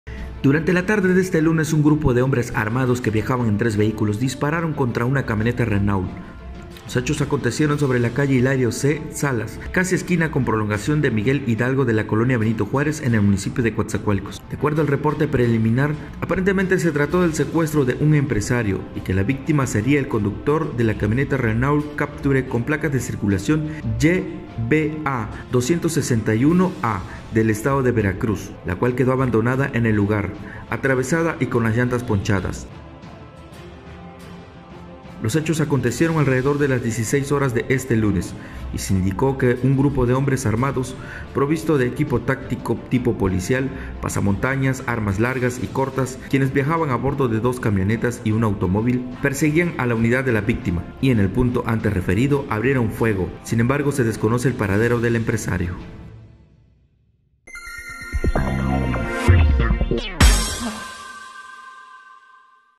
“Levantan” a empresario en Coatzacoalcos
​Este lunes poco después de las 16:00 horas vecinos de la colonia Benito Juárez, de la ciudad de Coatzacoalcos, al sur de Veracruz, vivieron momento de pánico luego de que se registrara lo que reportaron como “una balacera” que dejó un saldo de un “levantado”.